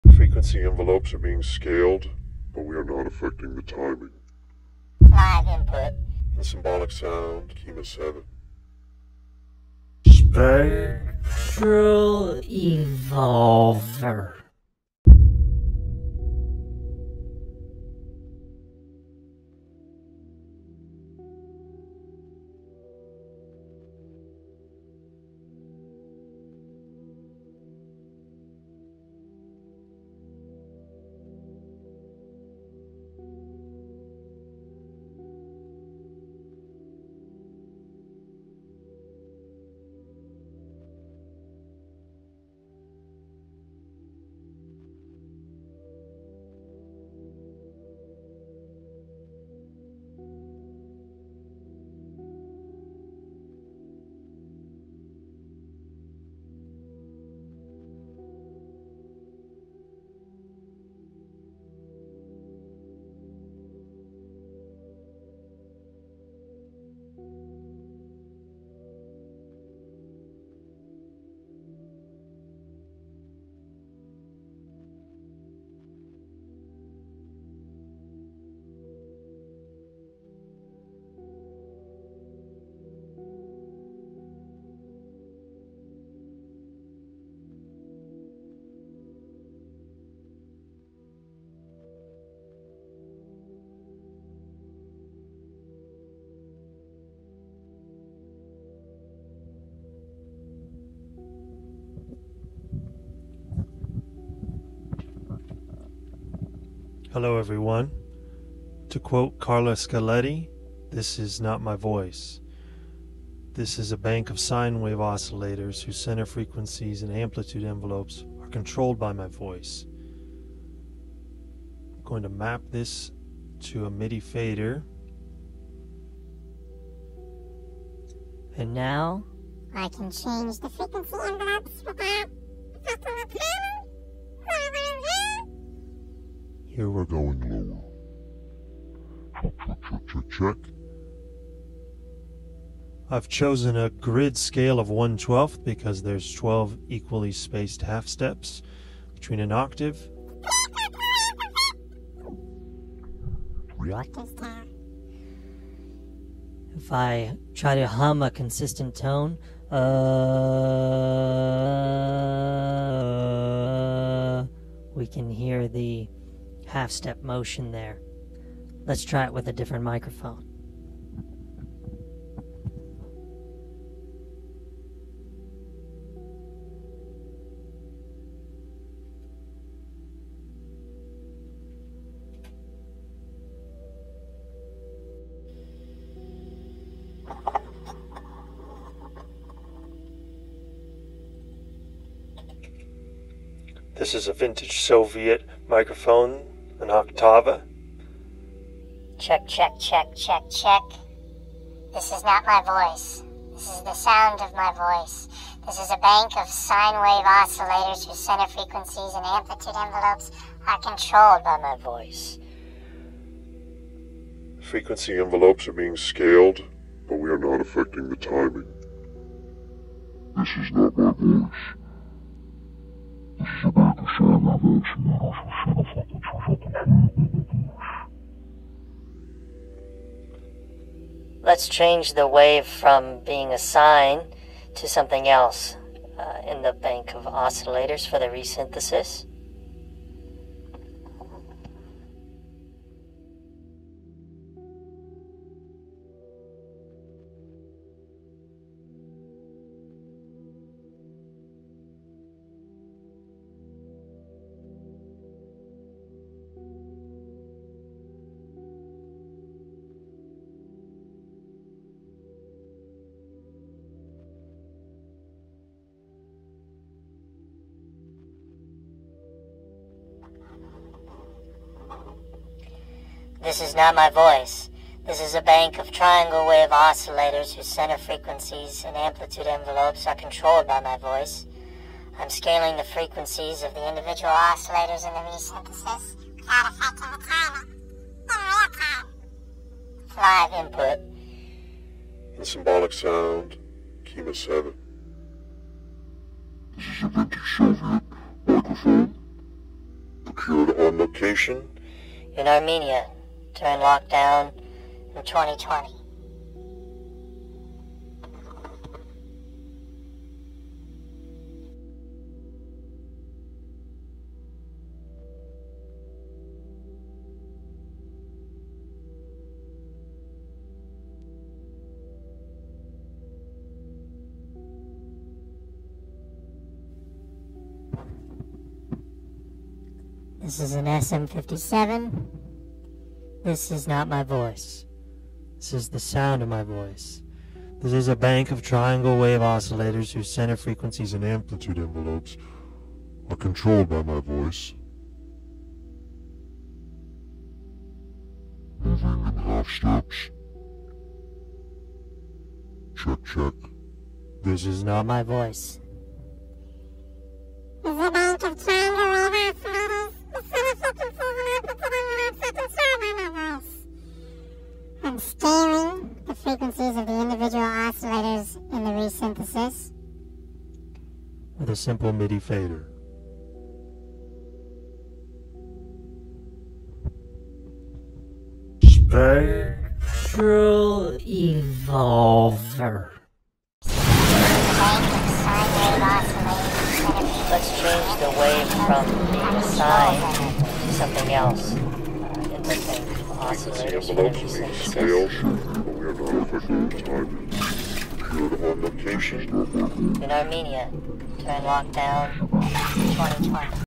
This is not my Voice | Kyma Patch from Scratch: Live Input Pitch Shifter Tutorial (Minimal Talking)
Among the many things Kyma excels at is spectral analysis and resynthesis, even in a live-input situation. Here I build a Kyma Sound which takes live-input from a microphone, independently extracts the frequency and amplitude envelopes for individual manipulation, then uses a bank of oscillators to resynthesize, in real-time, the sound from the spectral information. Having decoupled frequency and time we can do arithmetic on one without affecting the other. For example, in this video I scale the frequency envelopes by half-step divisions of the traditional octave, which I control with a simple MIDI fader, without changing the timing of the live-input.

Any specific questions just ask!

Quip Jar or try the 'SUPER THANKS' right here on YouTube.